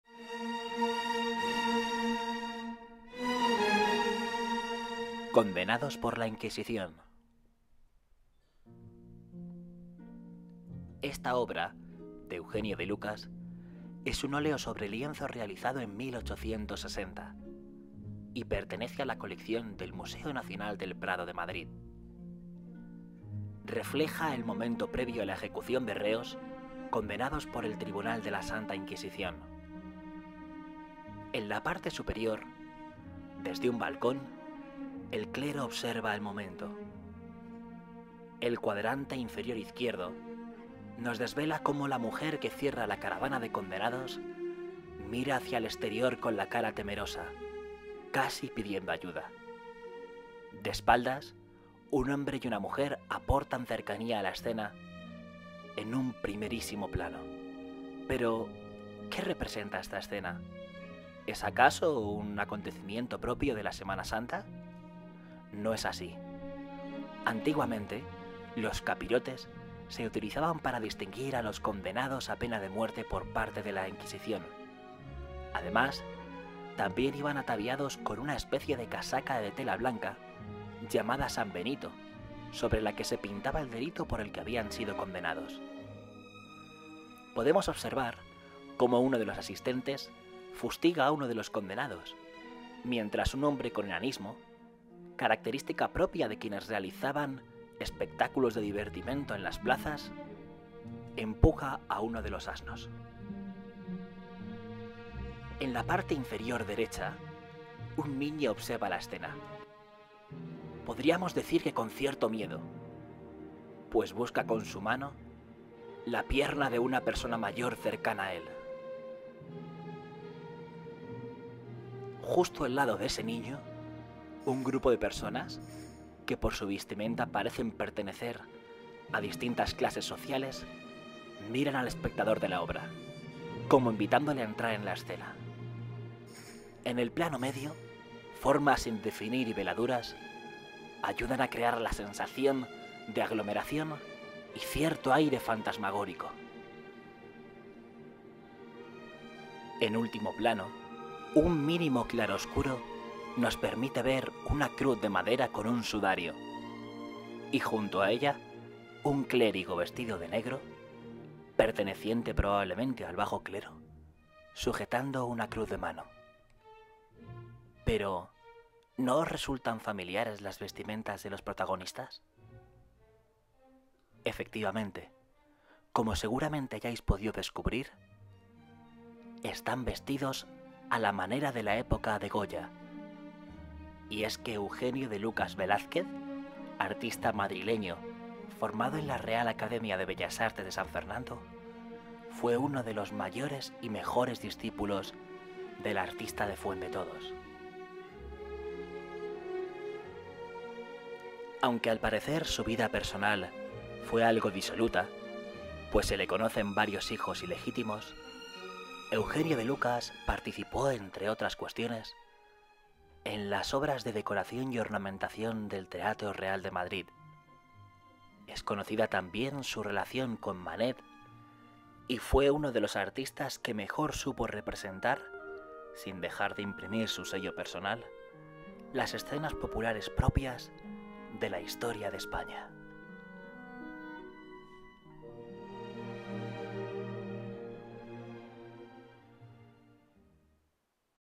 CONDENADOS POR LA INQUISICIÓN - EUGENIO DE LUCAS VELÁZQUEZ
Explicación sobre la obra "Condenados por la Inquisición", realizada por Eugenio de Lucas Velázquez y perteneciente a la colección del Museo Nacional del Prado de Madrid.

©Imagen: Museo Nacional del Prado.

Actividad creada por Juan Carlos Antón Rosales, Máster Universitario en Formación del Profesorado de Educación Secundaria Obligatoria, Bachillerato, Formación Profesional, Enseñanza de idiomas y Enseñanzas Deportivas dentro de la especialidad Geografía e Historia.